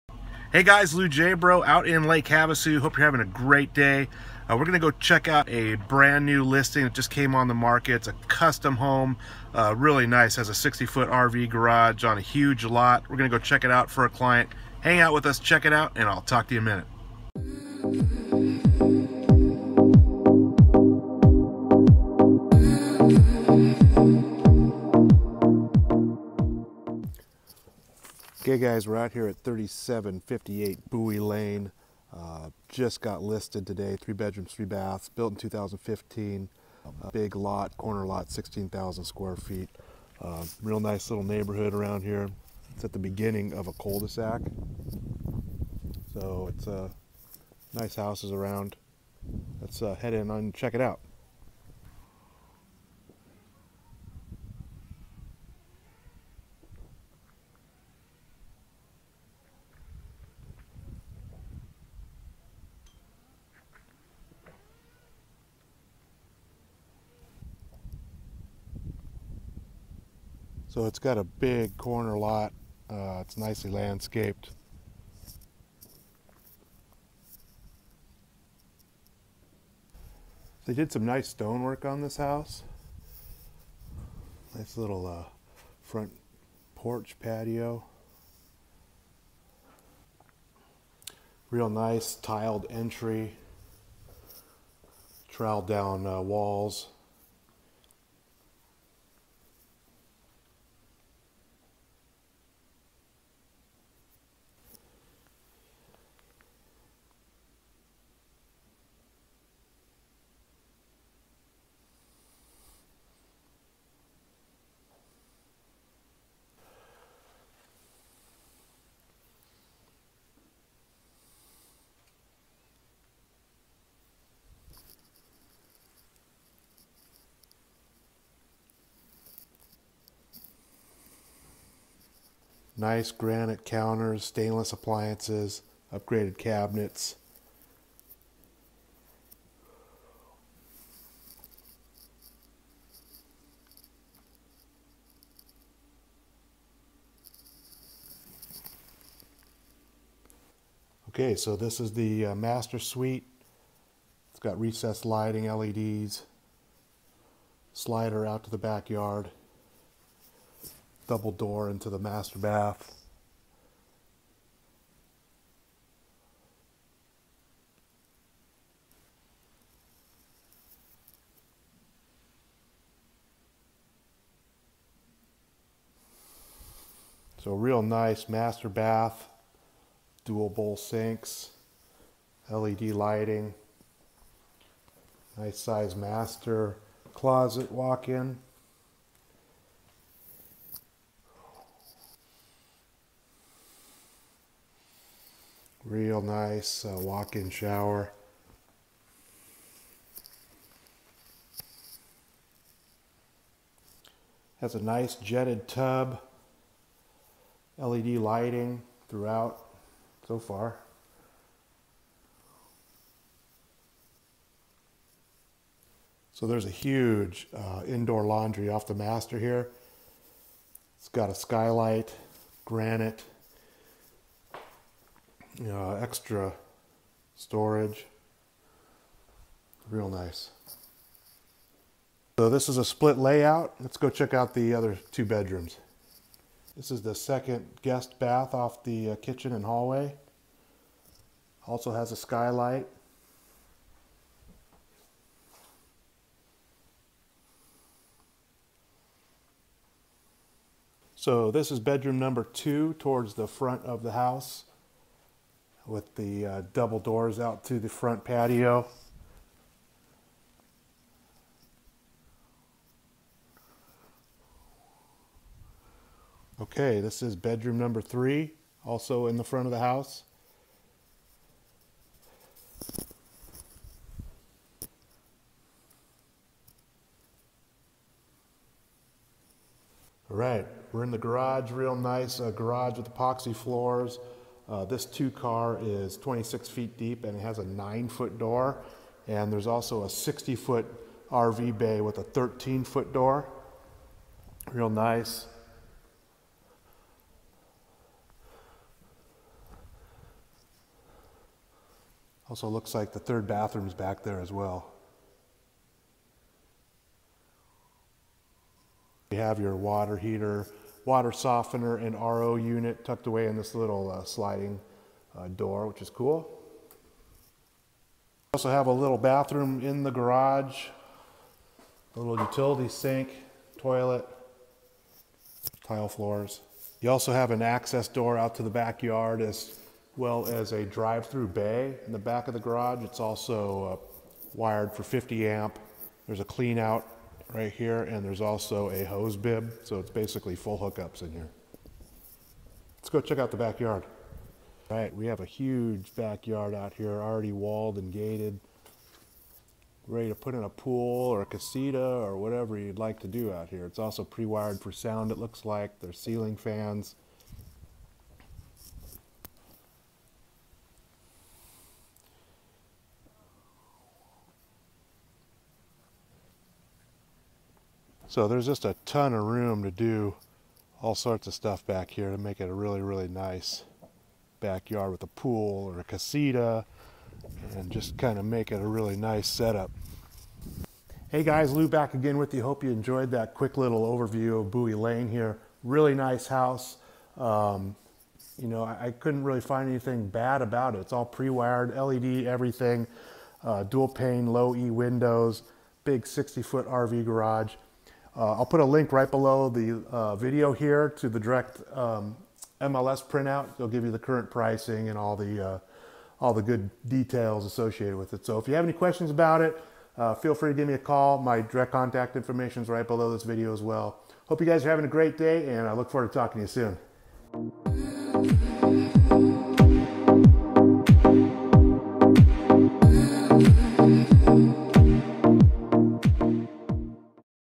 Lake Havasu RV Garage homes 3758 Buoy Ln MLS 923464
for complete listing information, current pricing and more pictures.

This is still "like new" in almost perfect condition. It must have been a part time home here in Lake Havasu. It is a 3/3 with huge 60' drive-thru RV garage, split floor plan, all the upgrades and sits on a huge corner lot in a nice neighborhood.

Have questions and just want to talk to a real person?

Not what you are looking for? See ALL Lake Havasu real estate for sale